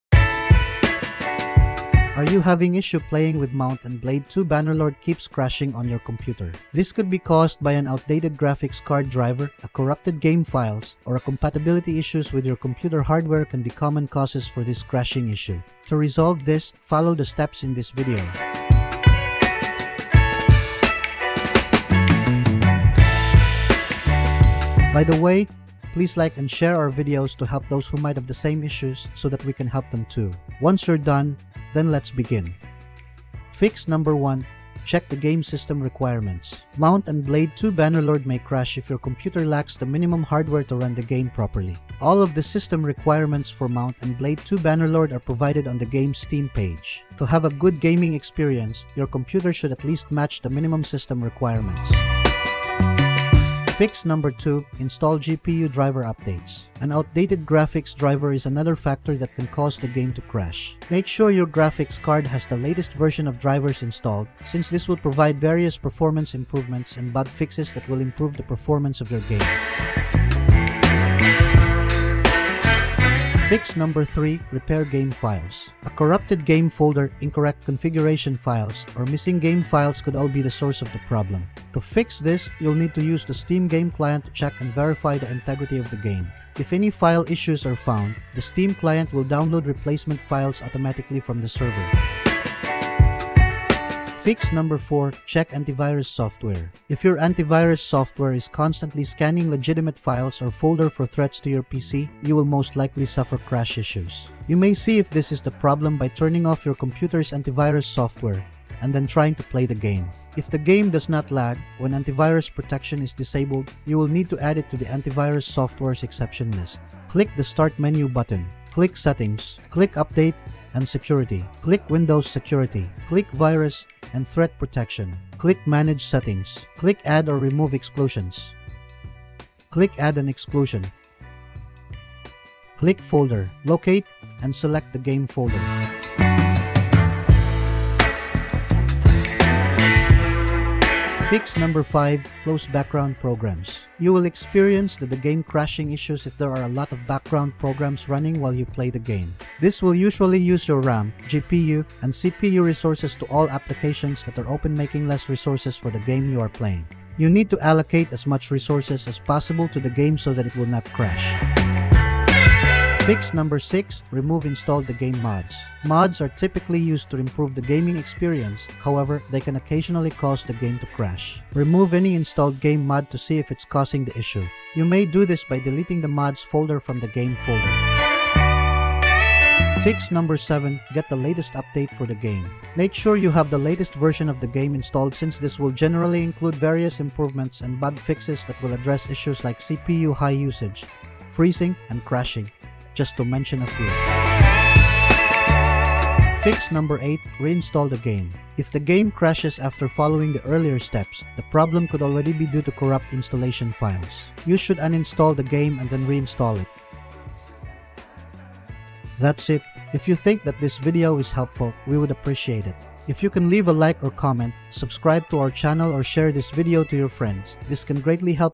How To Fix Mount and Blade 2: Bannerlord Keeps Crashing On PC [Updated 2024]
Are you having issue playing with Mount & Blade 2 Bannerlord keeps crashing on your computer? This could be caused by an outdated graphics card driver, a corrupted game files, or a compatibility issues with your computer hardware can be common causes for this crashing issue. To resolve this, follow the troubleshooting steps in this video.